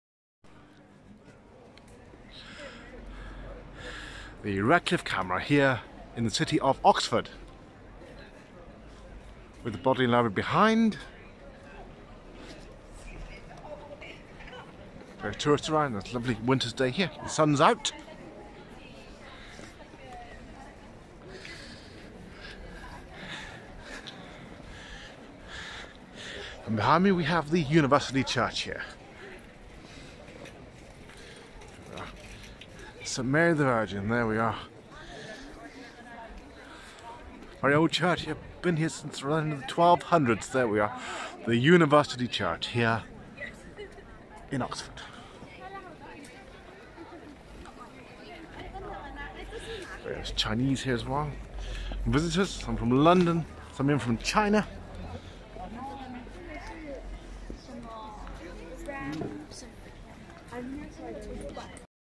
Radcliffe Camera, Oxford
Shows the iconic Radcliffe Camera building in the centre of Oxford